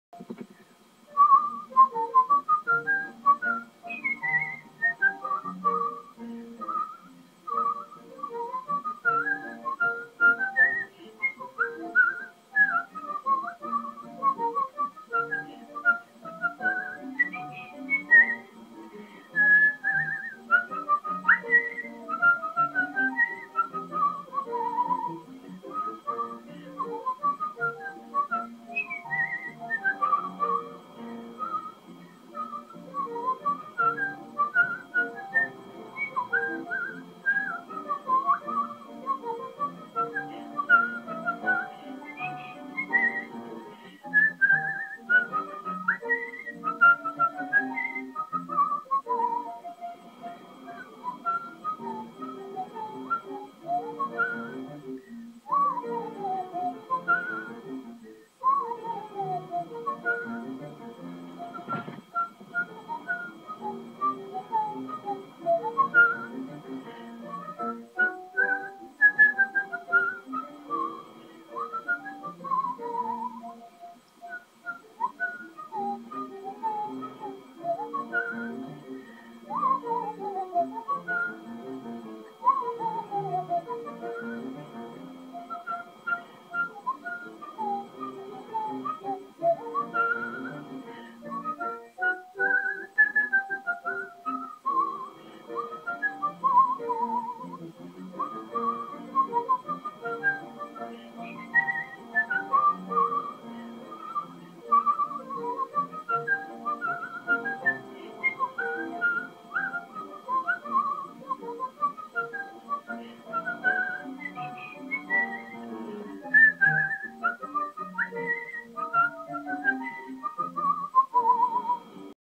Part of a ragtime piece
Part of a ragtime piece. I'm a little rusty, but I'm returning to recording whistling videos to upload to YouTube.com.